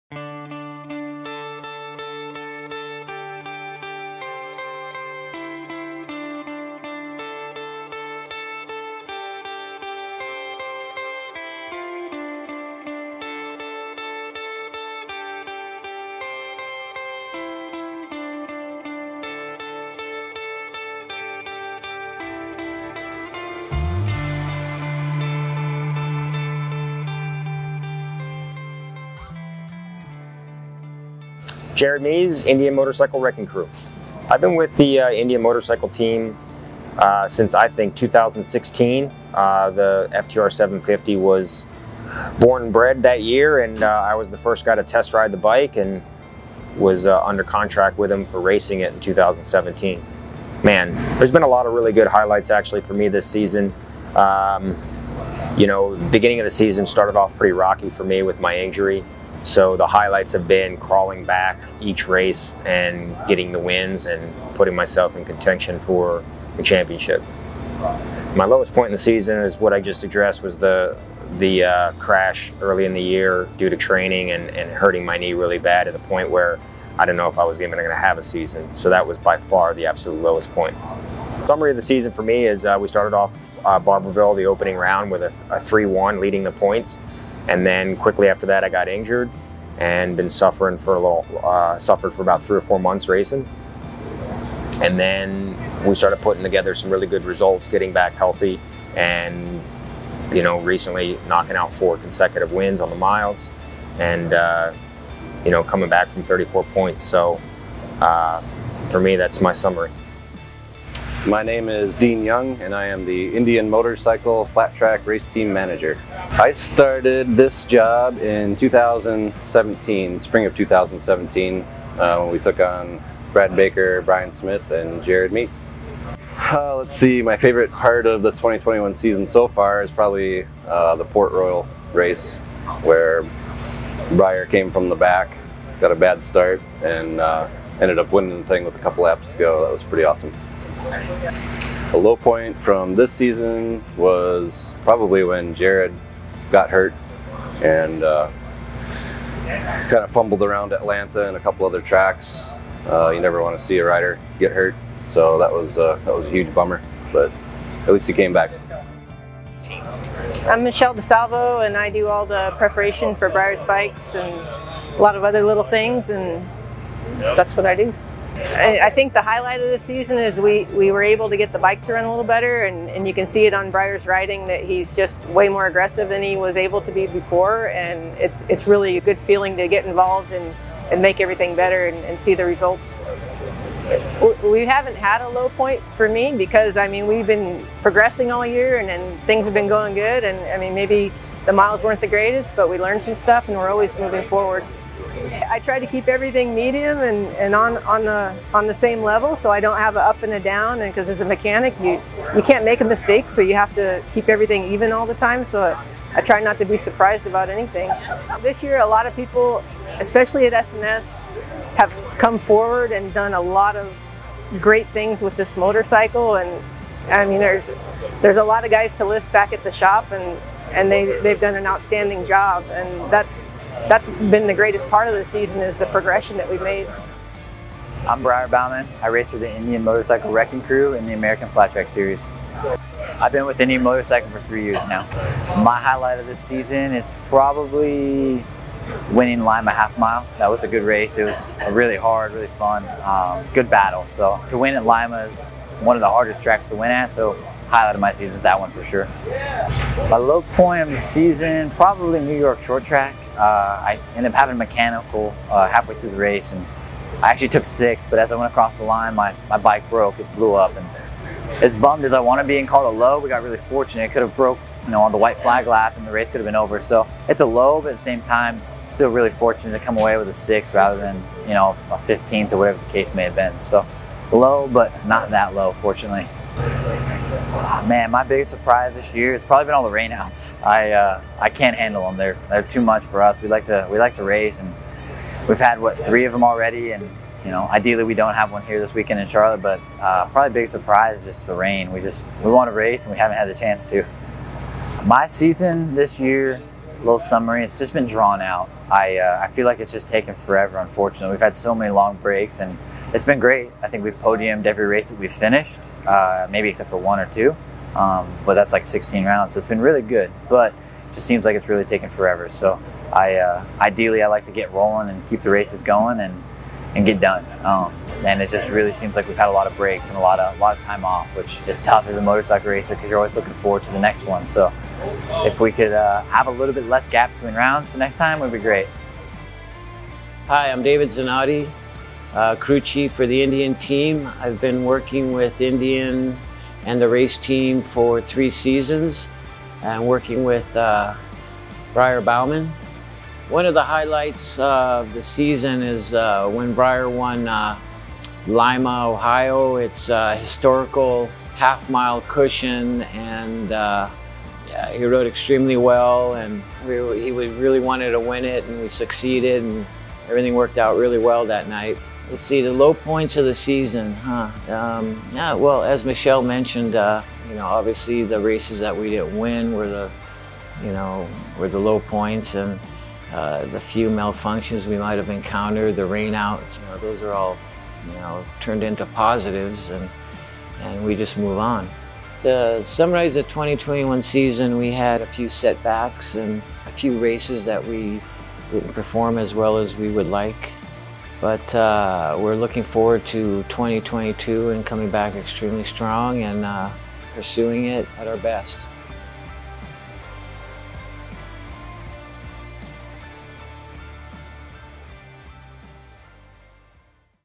AFT 2021 Season in Review
2021 was an epic season for the S&S supported Indian Wrecking Crew. Lots of ups and downs, some injuries, and a fair amount of mid-season rule changes. Special thank you's to racers Briar Bauman, Jared Mees, and the team including Dean, Dave, Michelle and Kevin. Some exciting stuff already planned for 2022!